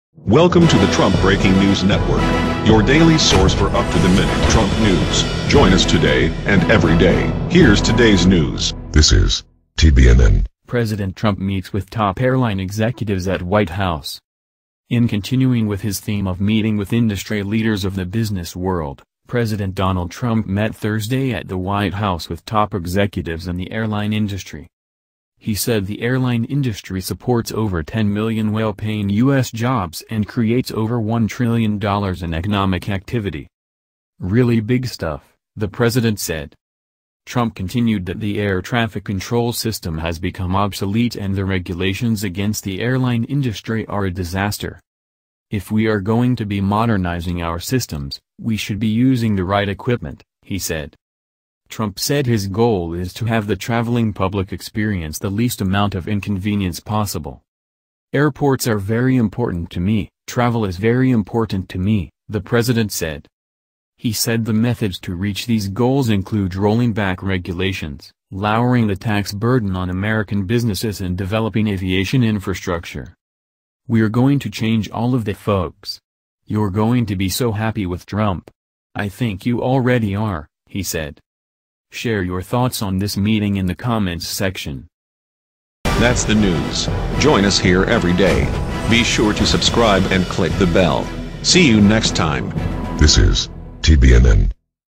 Pres Trump Meets with Top Airline Executives at White House
Trump Breaking News Network -Pres. Trump Meets with Top Airline Executives at White House.

In continuing with his theme of meeting with industry leaders of the business world, President Donald Trump met Thursday at the White House with top executives in the airline industry.

He said the airline industry supports over 10 million well-paying U.S. jobs and creates over $1 trillion in economic activity.

"Really big stuff," the president said.

Trump continued that the air traffic control system has become obsolete and the regulations against the airline industry are a "disaster."

"If we are going to be modernizing our systems, we should be using the right equipment," he said.

Trump said his goal is to have the traveling public experience the least amount of inconvenience possible.

"Airports are very important to me; travel is very important to me," the president said.

He said the methods to reach these goals include rolling back regulations, lowering the tax burden on American businesses and developing aviation infrastructure.

"We're going to change all of that folks. You're going to be so happy with Trump. I think you already are," he said.

Share your thoughts on this meeting in the comments section.

Category
News & Politics
License